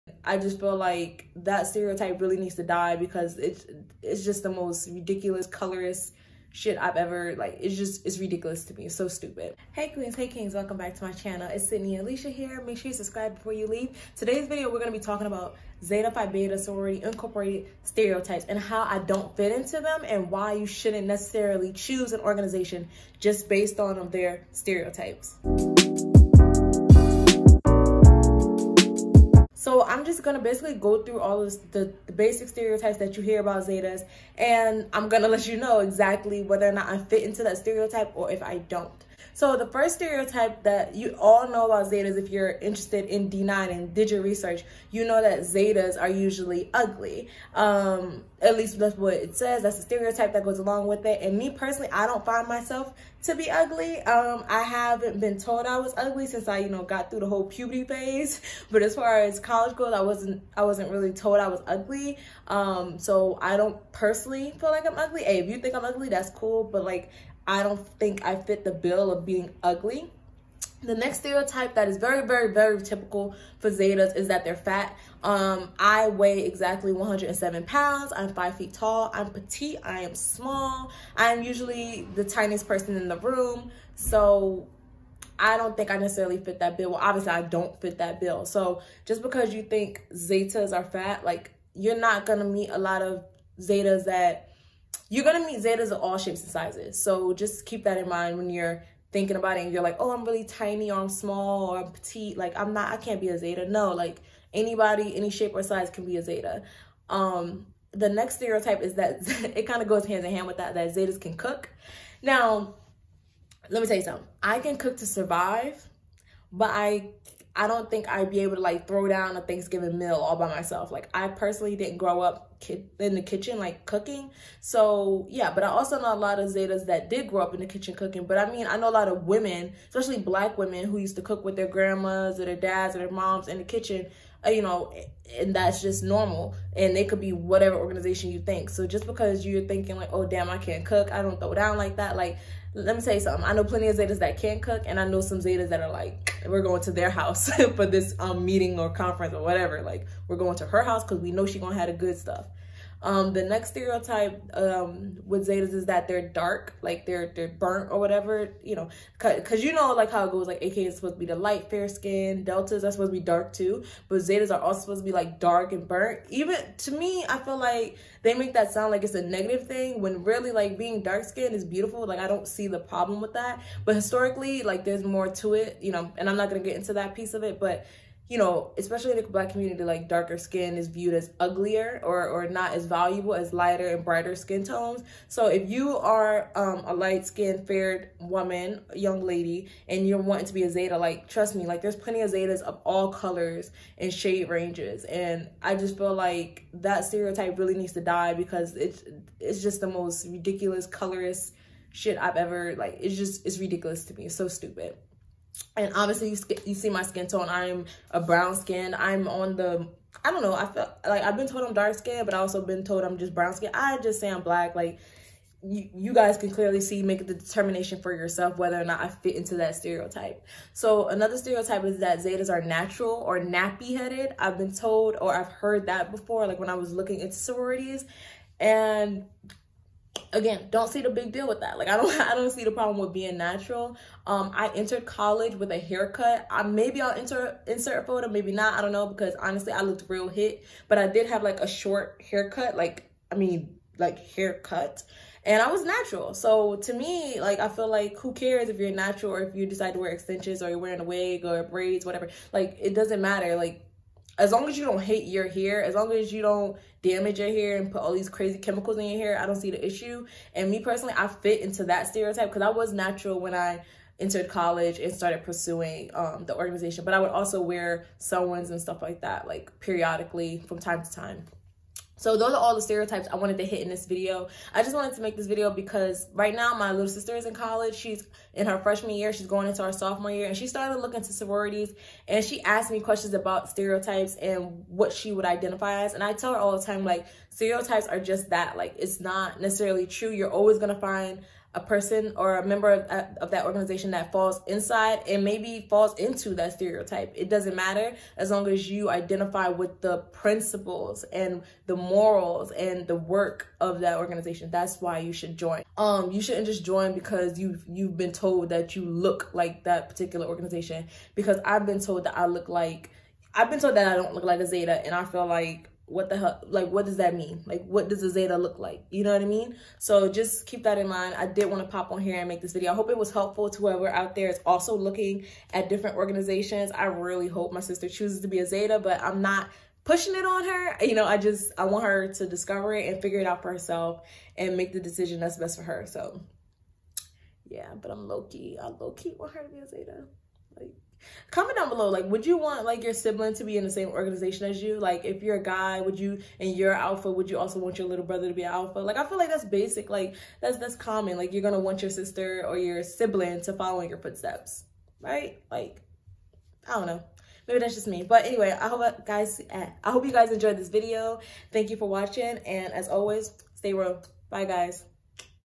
Am I A "REAL" Zeta?! | Zeta Phi Beta Stereotypes! | Colorism
Hey There. Todays video will be discussing some of the main stereotypes I heard and have been told about Zetas.

Comment any stereotypes that you have heard that I missed!

Beauty Essentials Soap Co.